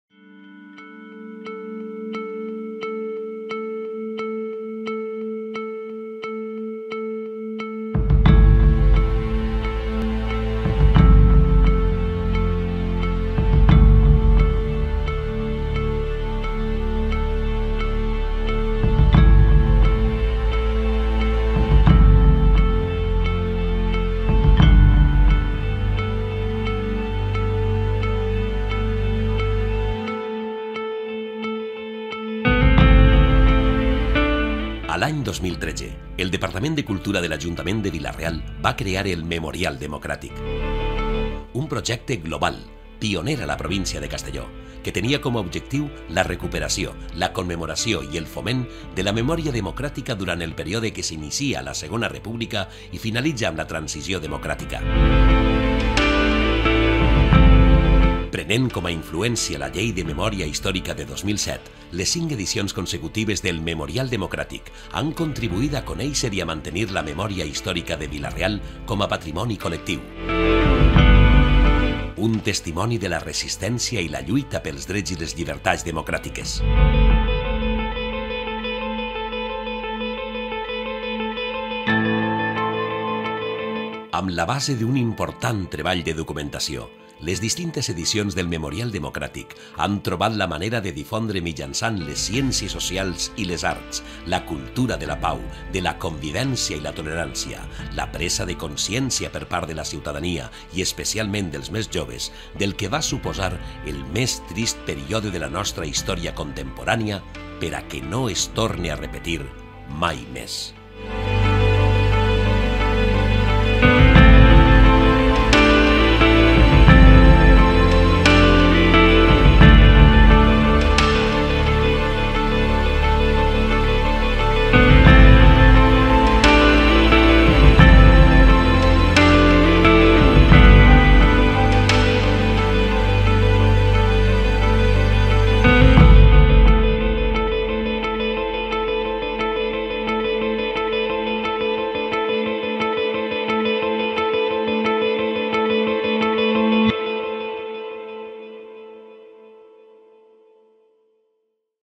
5 anys del Memorial Democràtic a Vila real (2013-2018)
Recull d'imatges amb motiu dels 5 anys del Memorial Democràtic a Vila-real, un projecte de la Regidoria de Cultura per tractar de donar a conèixer la història i que no es torne a repetir.